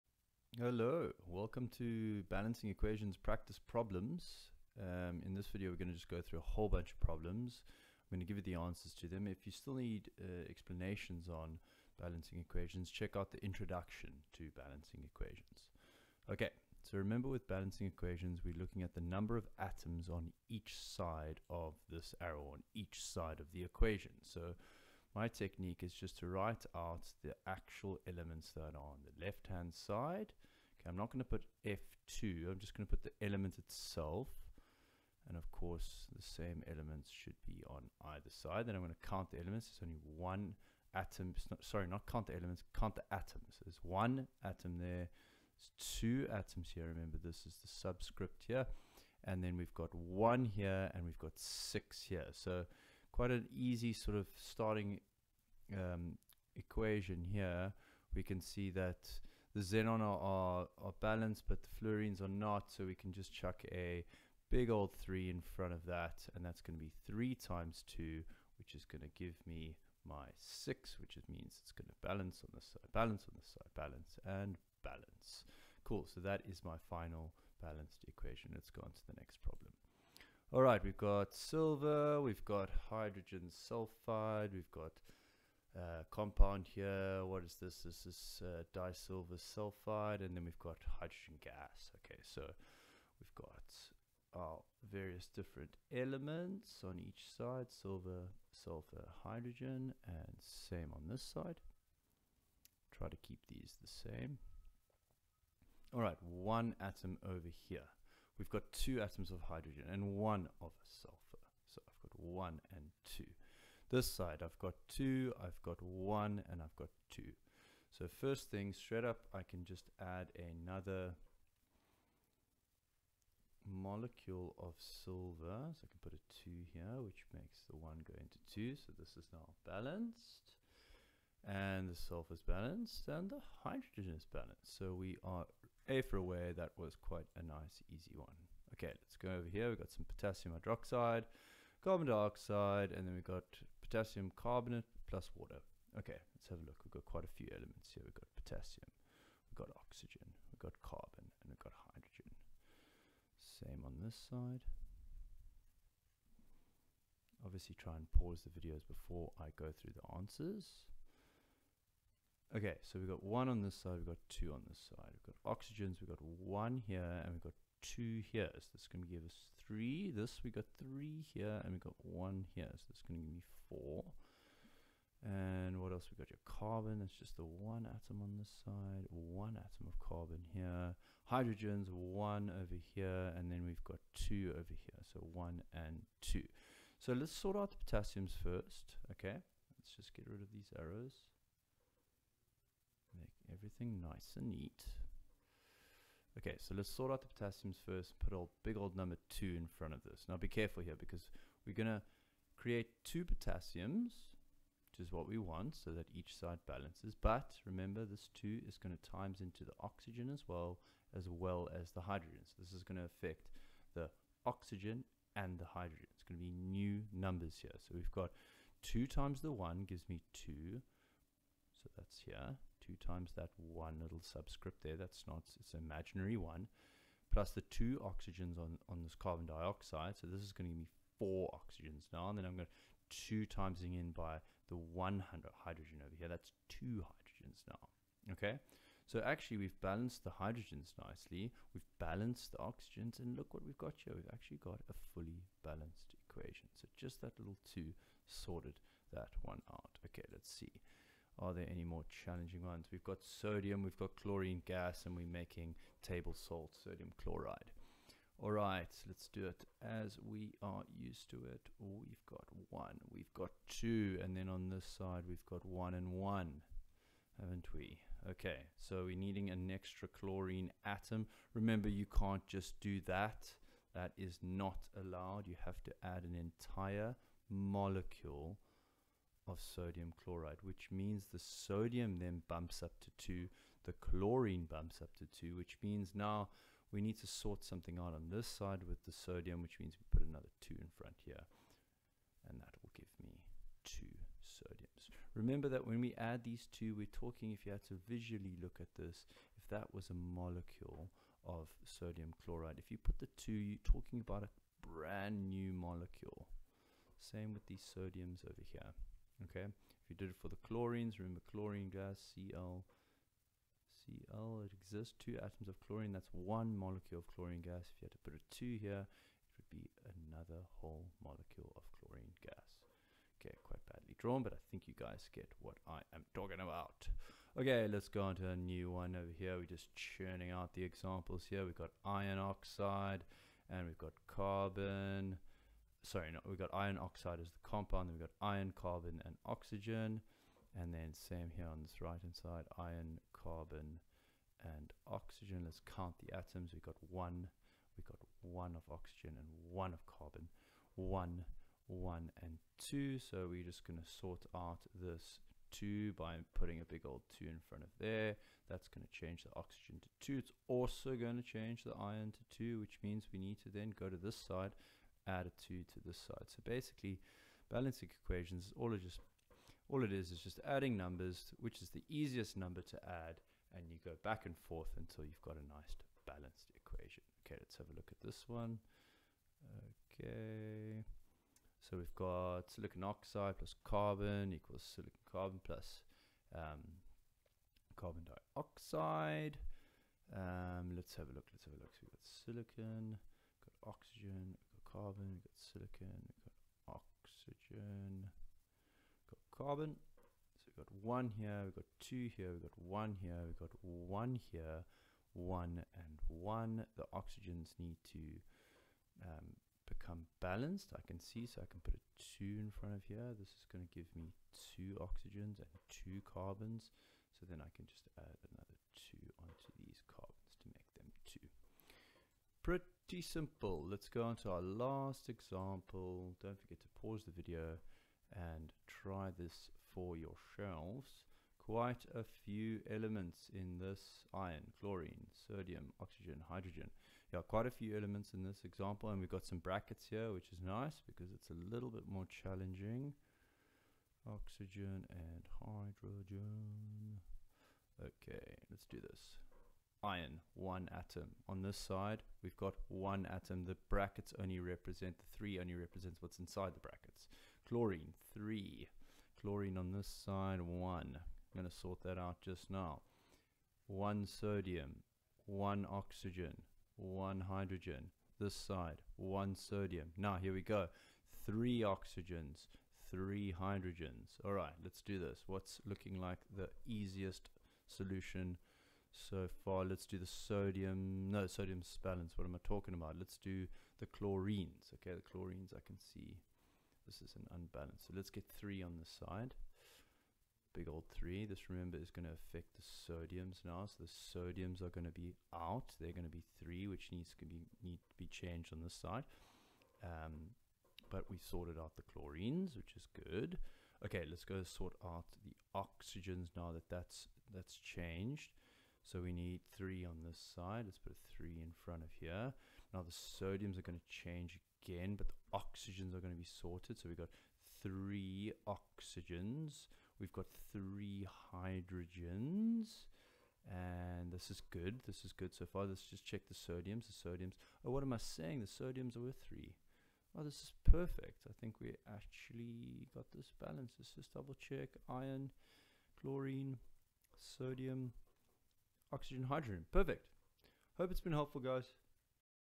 Int GCSE Chemistry M7 S2 Screencast 2
In this video, you will go thorugh some practice problems on balancing equations.